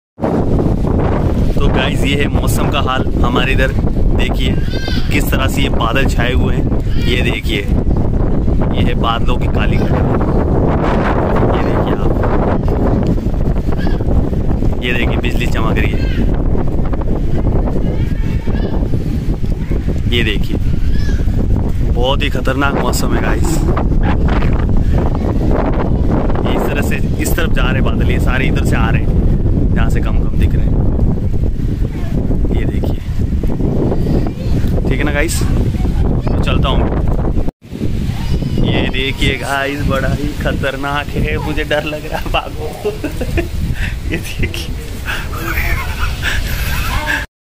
ऐसी कड़कड़ाती बिजली आपने कहीं नहीं देखी होगी
जबरदस्त तूफान बाड़मेर में